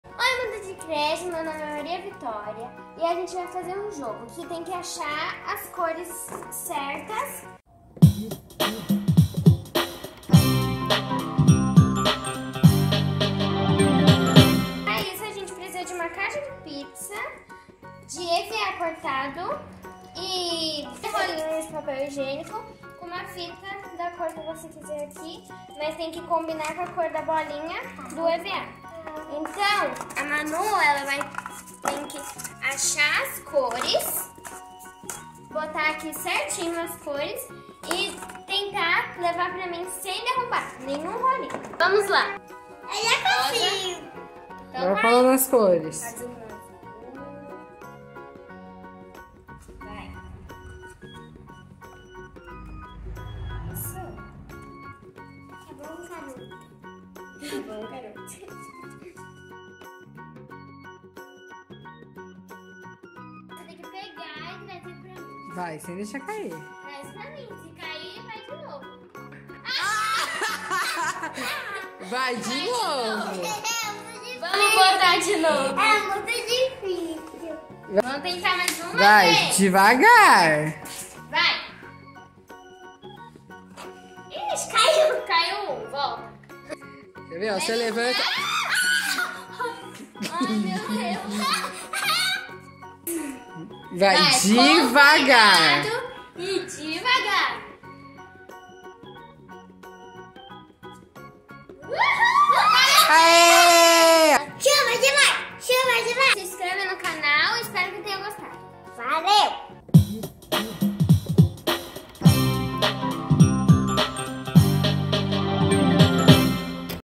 DESAFIO DAS CORES | EDUCAÇÃO INFANTIL | CONCENTRAÇÃO | PERCEPÇÃO VISUAL
Todo mundo tem em casa rolinhos de papel higiênico certo?

Que tal guardar alguns e confeccionar um brinquedo super divertido  para as crianças?
Com esta atividade, trabalhamos a cres, percepção visual, motricidade fina, concentração  atenção.
No final da proposta ainda podemos inventar mais desafios, como a Manu inventou de levar uma bandeja com rolihos sem cair!

Poste aqui seu tabuleiro do desafios das cores !

Bom divertimento!